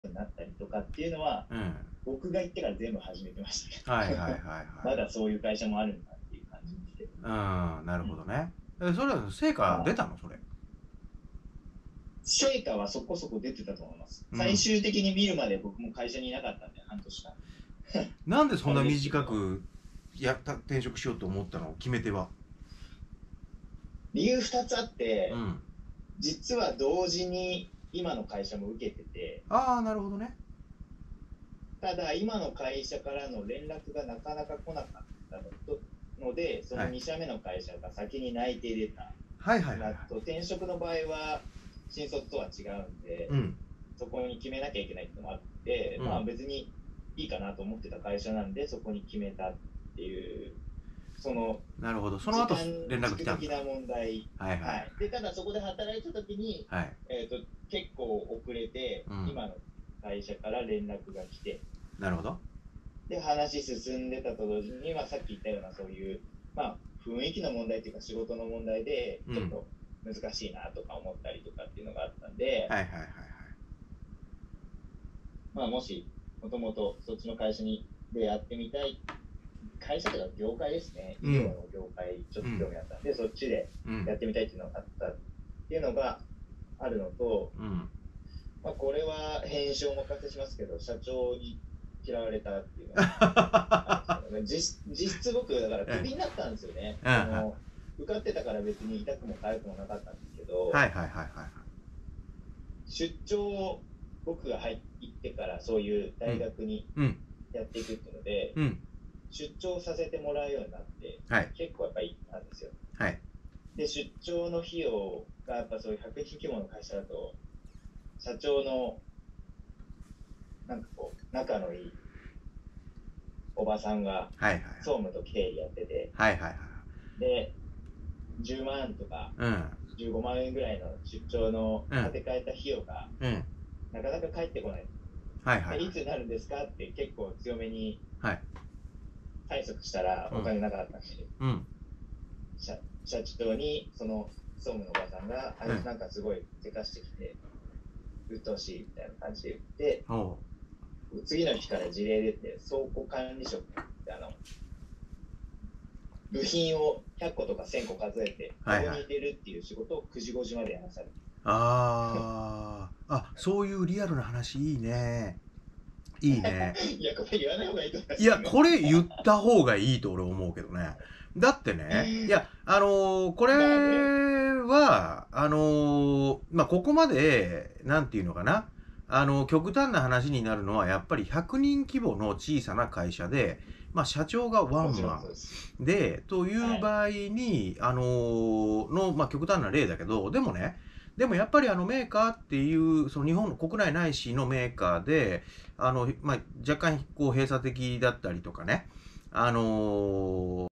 【代わりにOBOG訪問2-4】複数の業界と職種を比較（営業/小売→人事/小売→人事/内資機械メーカー→営業/外資系医療機器メーカー）/社会人10年目/男性に仕事インタビュー
複数業界複数職種比較バージョン「REAL JOB TALK」part.4。

小売の営業→小売の人事→機械メーカーの人事→外資系医療機器メーカーの営業。3社2職種の経験をもとに会社雰囲気や仕事内容について、【業界による違い】、【内資と外資の違い】、【仕事内容の違い】がわかるように聞いてみました。
このタイトルはpart1～part9に分けて公開しています。
part1から順番に視聴すると理解しやすいです。